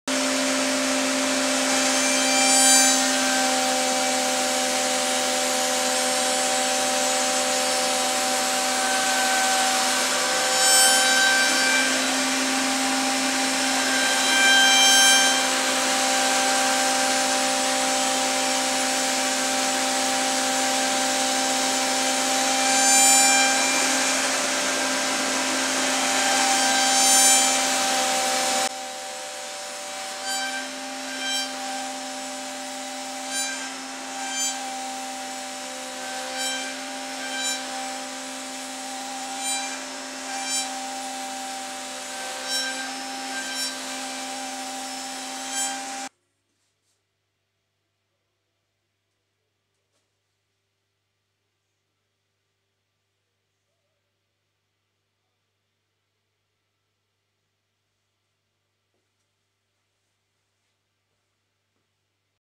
'Planing' oak on the cnc router
This piece of oak I need to be parallel to the machine before doing more work on it.
Using a 60mm spoilboard cutter to 'plane' it. Before having this mill I used a 16mm mill, the time saving with this bigger mill is substantial.
The router is only capable of 1.5m/min at the moment, faster feed rate would be beneficial for this sort of cutting.
The 2.2 kw spindle has only 'just' about enough torque to make this work in oak using a small cutting depth.